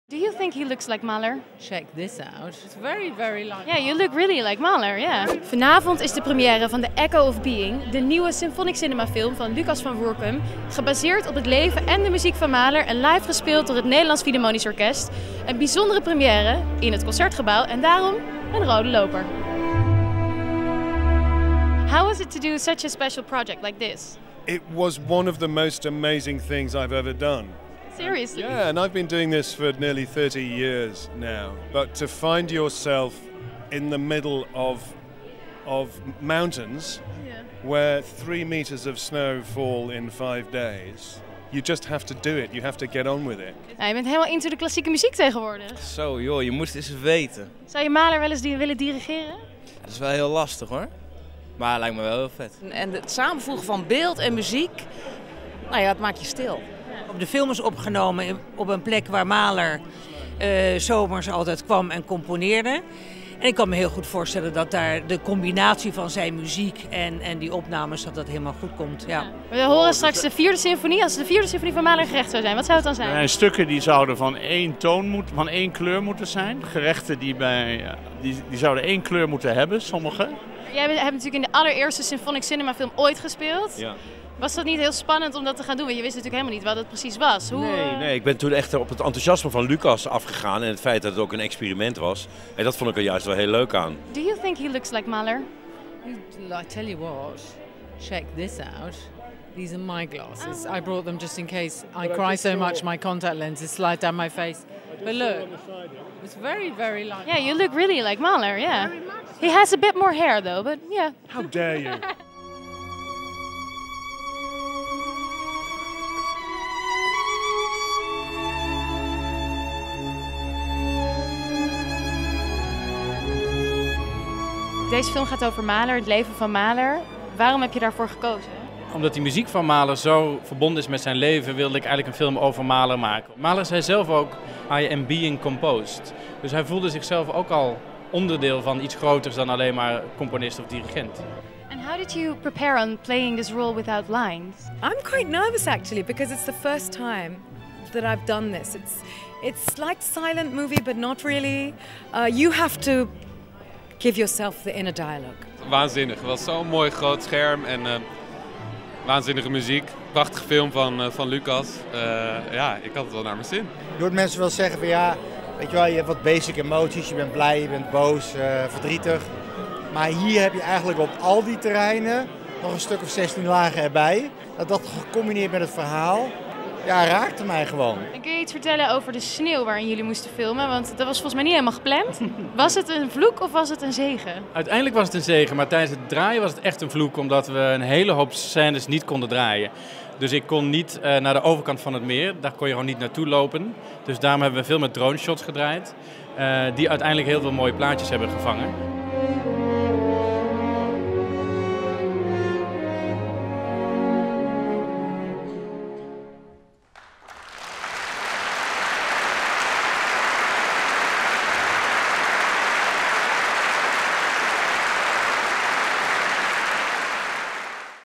Première The Echo of Being: het leven en de muziek van Mahler verfilmd
De Symphonic Cinema film 'The Echo of Being' ging deze week in première! Gebaseerd op het leven van Mahler en met zijn muziek als live soundtrack kon dat natuurlijk nergens anders plaatsvinden dan in Het Concertgebouw. Iris van der Valk zocht uit of hoofdrolspeler Greg Wise eigenlijk wel op Mahler lijkt en of de sneeuw een vloek of een zegen was.

De muziek werd uitgevoerd door het Nederlands Philharmonisch Orkest onder leiding van dirigent Antony Hermus.